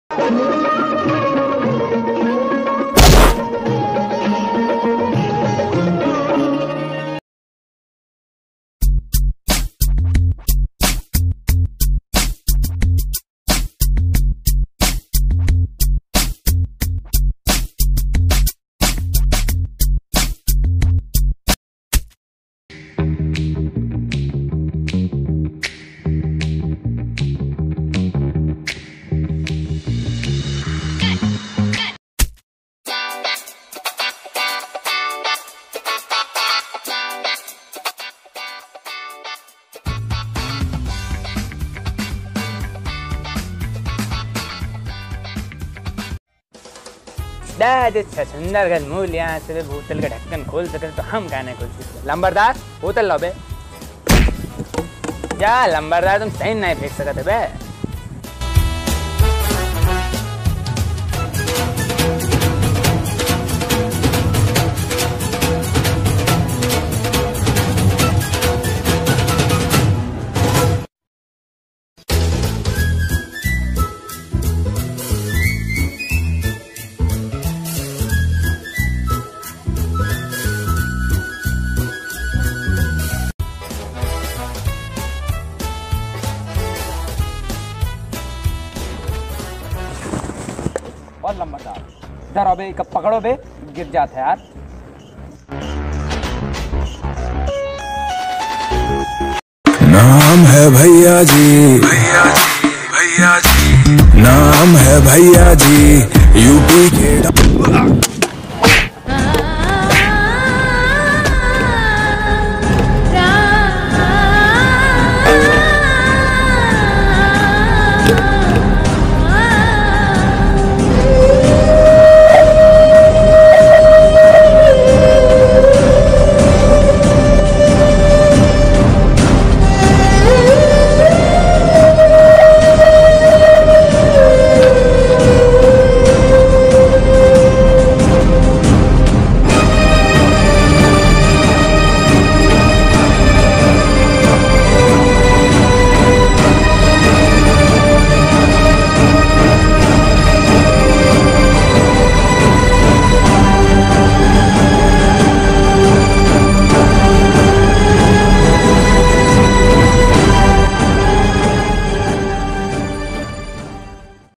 What happened when U.P. guy did the Bottle Cap Challenge
About ; In this we tried to the viral and trending challenge The Bottle Cap Challenge
Our character Bhaiyaji who is from U.P. tried to did the Bottle Cap challenge
In this video you will see what happened when A u.p. guy tried to do the bottle cap challenge.
It is a funny sketch video .